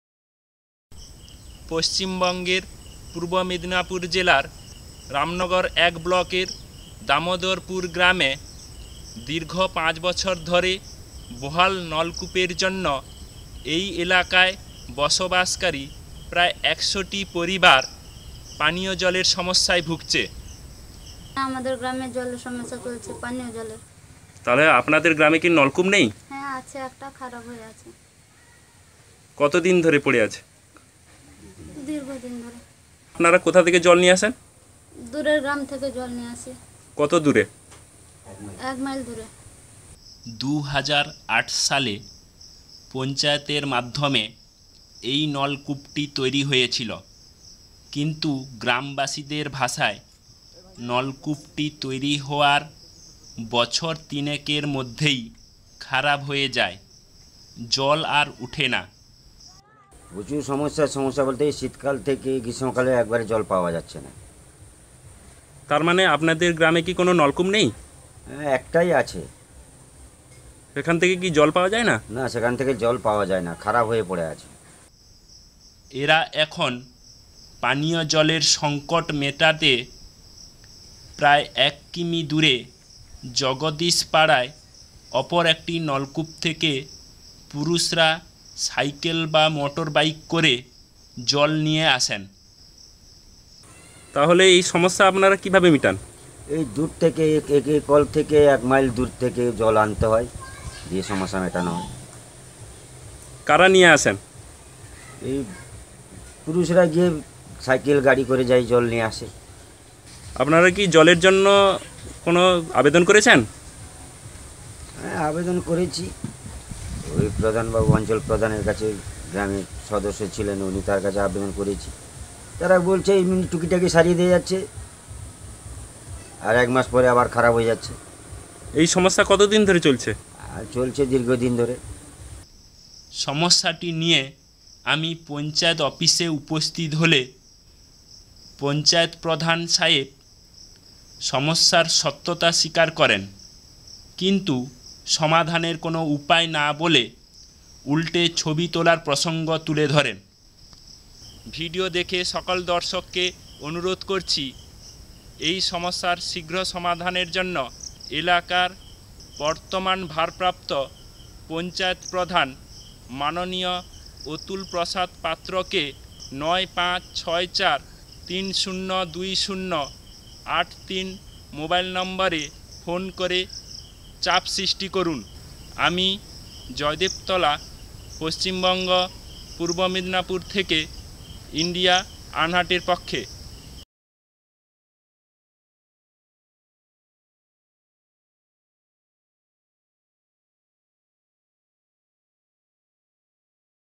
Residents suffer from water related issues for 5 years
Jaydeb Tala reports from East Midnapur, West Bengal for Video Volunteers.

iCountryRide
coutryridei